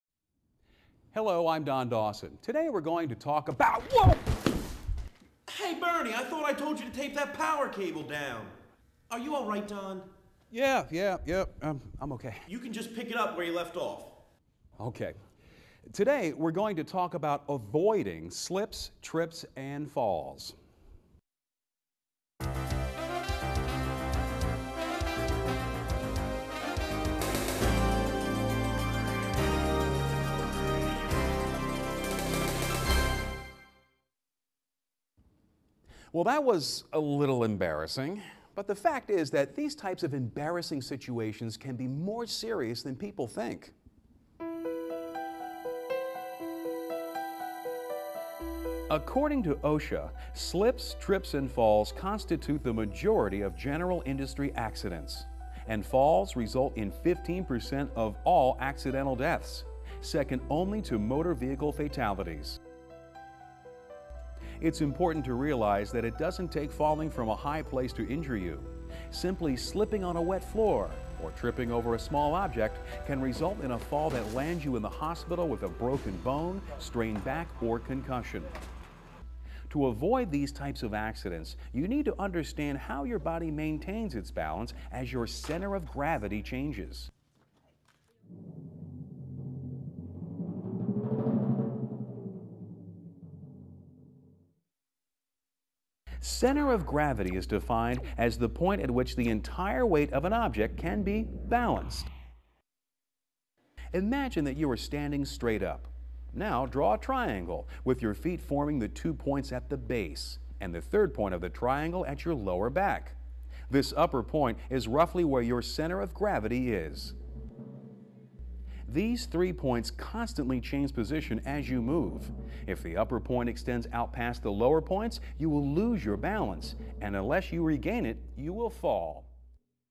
Slip, Trip and Fall Hazards | Slip Trip and Fall Prevention
You can slip when you lose your footing, you can trip when you catch your foot on or in something, and you fall when you come down suddenly. Spills, ice, snow, rain, loose mats, rugs, and stepladders are some of the common causes of slips, trips, and falls.